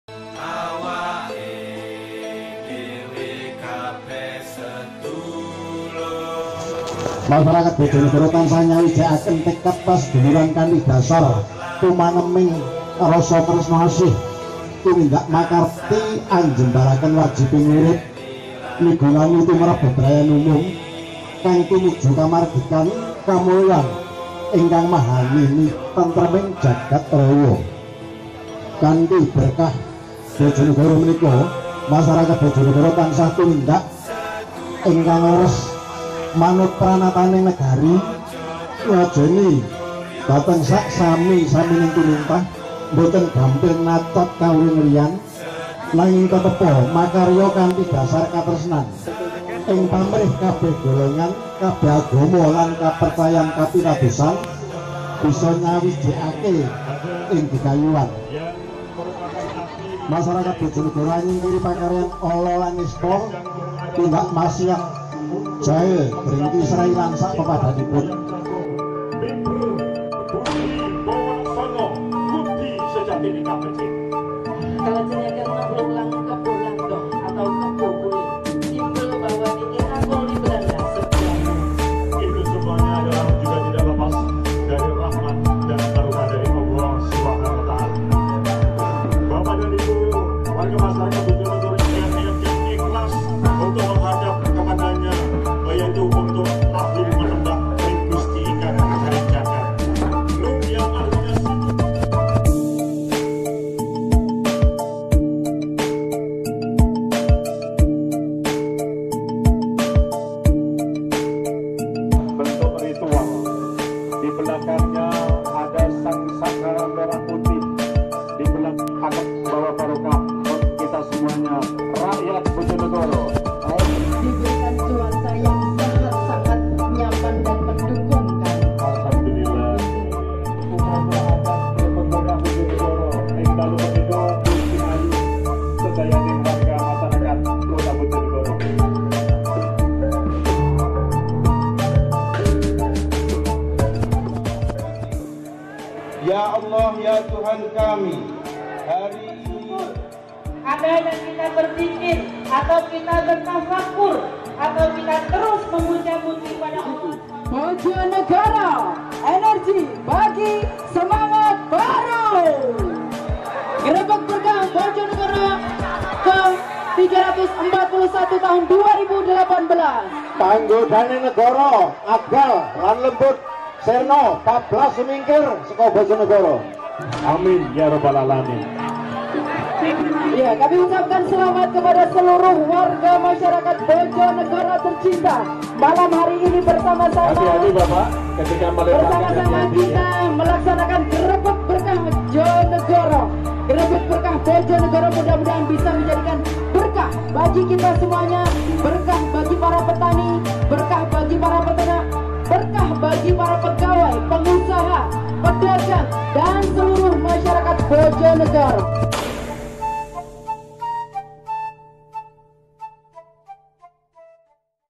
Grebeg Berkah Bojonegoro
Grebeg Berkah Bojonegoro merupakan festival kebudayaan yang diselenggarakan untuk memperingati hari jadi kabupaten Bojonegoro. Acara ini digelar di beberapa titik seperti area Kahyangan Api, Pendapa Malowopati dan Alun-alun kota Bojonegoro.